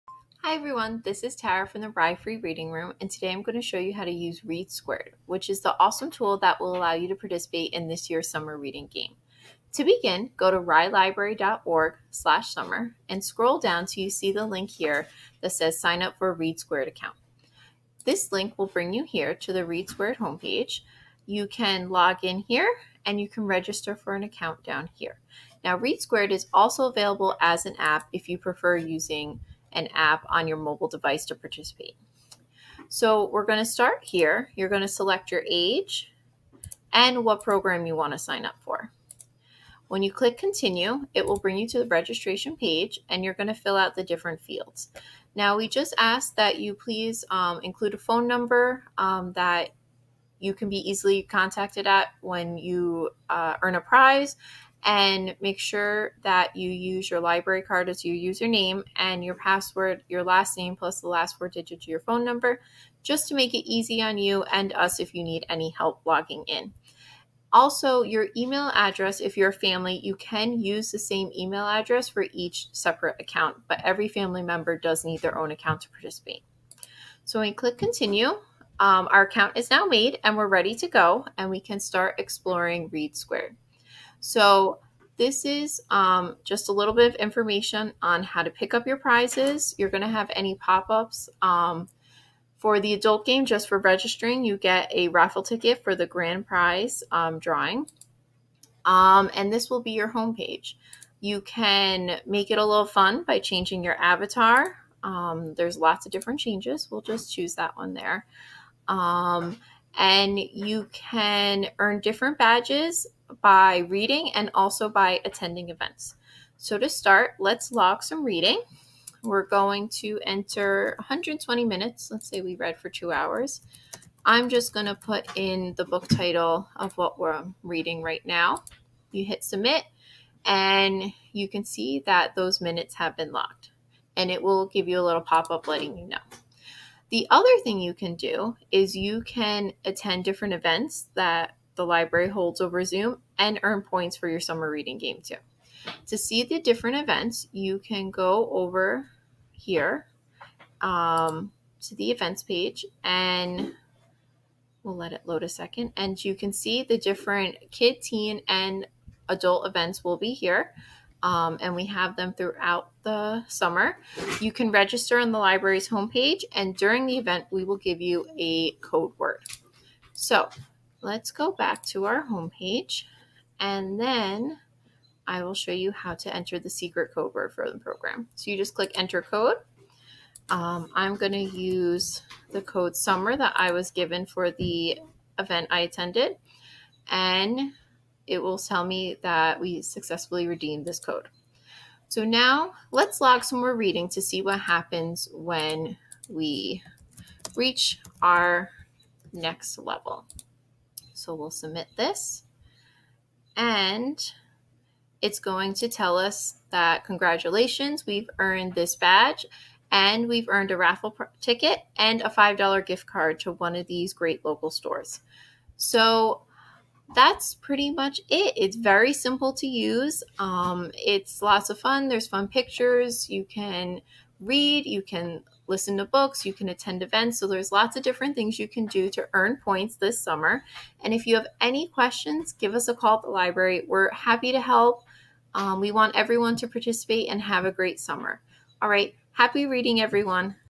READsquared
Learn how to navigate the READsquared website to participate in our Summer Reading Game!